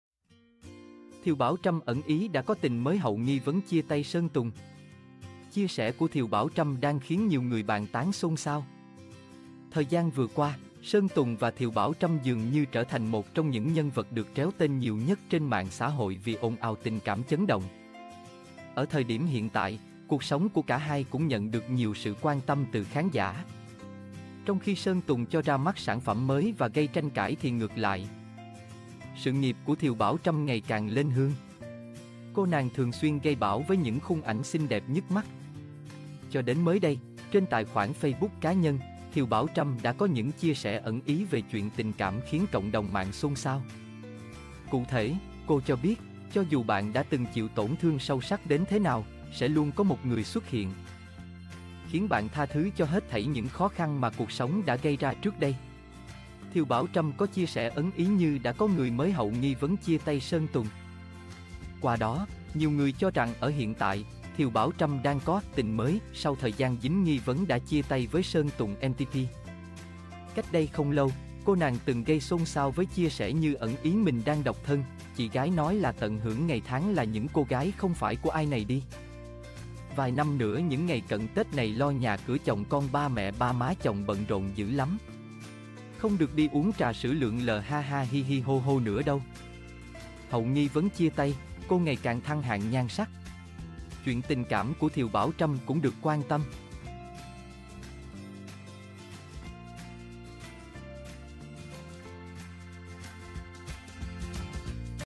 Thiều Bảo Trâm ẩn ý đã có tình mới hậu nghi vấn chia tay Sơn Tùng?
Chia sẻ của Thiều Bảo Trâm đang khiến nhiều người bàn tán xôn xao.

Thời gian vừa qua, Sơn Tùng và Thiều Bảo Trâm dường như trở thành một trong những nhân vật được réo tên nhiều nhất trên mạng xã hội vì ồn ào tình cảm chấn động. Ở thời điểm hiện tại, cuộc sống của cả 2 cũng nhận được nhiều sự quan tâm từ khán giả. Trong khi Sơn Tùng cho ra mắt sản phẩm mới và gây tranh cãi thì ngược lại, sự nghiệp của Thiều Bảo Trâm ngày càng lên hương. Cô nàng thường xuyên gây bão với những khung ảnh xinh đẹp nhức mắt.

Cho đến mới đây, trên tài khoản Facebook cá nhân, Thiều Bảo Trâm đã có những chia sẻ ẩn ý về chuyện tình cảm khiến cộng đồng mạng xôn xao. Cụ thể, cô cho biết: "Cho dù bạn đã từng chịu tổn thương sâu sắc đến thế nào, sẽ luôn có một người xuất hiện, khiến bạn tha thứ cho hết thảy những khó khăn mà cuộc sống đã gây ra trước đây".

Thiều Bảo Trâm có chia sẻ ấn ý như đã có người mới hậu nghi vấn chia tay Sơn Tùng
Qua đó, nhiều người cho rằng ở hiện tại, Thiều Bảo Trâm đang có "tình mới" sau thời gian dính nghi vấn đã chia tay với Sơn Tùng M-TP. Cách đây không lâu, cô nàng từng gây xôn xao với chia sẻ như ẩn ý mình đang độc thân: "Chị gái nói là tận hưởng ngày tháng là những cô gái không phải của ai này đi. Vài năm nữa những ngày cận Tết này lo nhà cửa chồng con ba mẹ ba má chồng bận rộn dữ lắm. Không được đi uống trà sữa lượn lờ ha ha hi hi hô hô nữa đâu".

Hậu nghi vấn chia tay, cô ngày càng thăng hạng nhan sắc. Chuyện tình cảm của Thiều Bảo Trâm cũng được quan tâm.